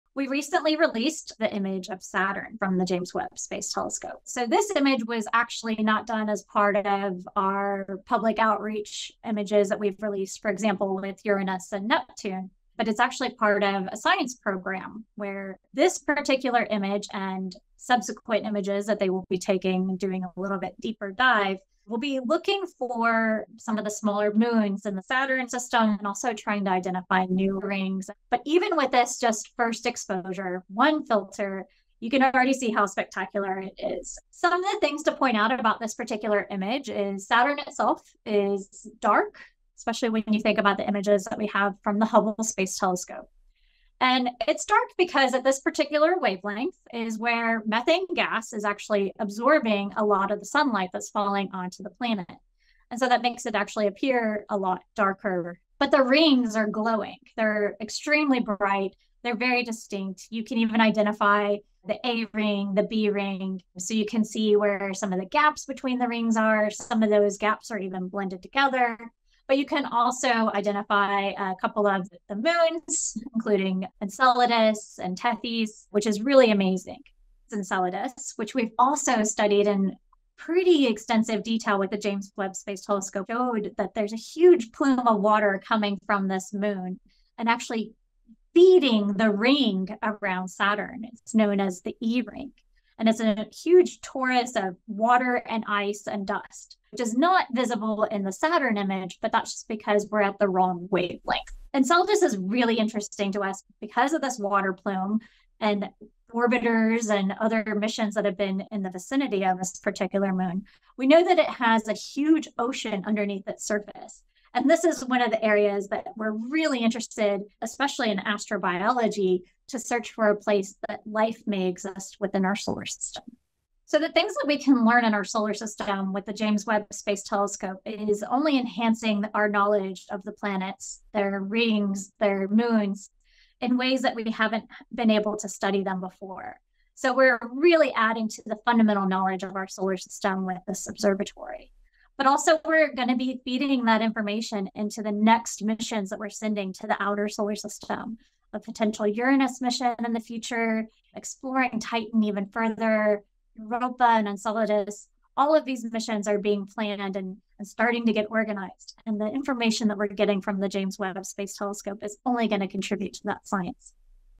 Saturn from the JWST 🪐
NASA Scientist Dr. Stefanie Milam dives deep into The James Webb Space Telescope's first near-infrared photo of Saturn. Witness the dark allure of Saturn itself, shrouded in methane gas, while its iconic rings, glowing brightly, steal the show. Stefanie Milam explains how the moon Enceladus could have biomarkers of life due to its hidden ocean and how JWST is helping us study Saturn further.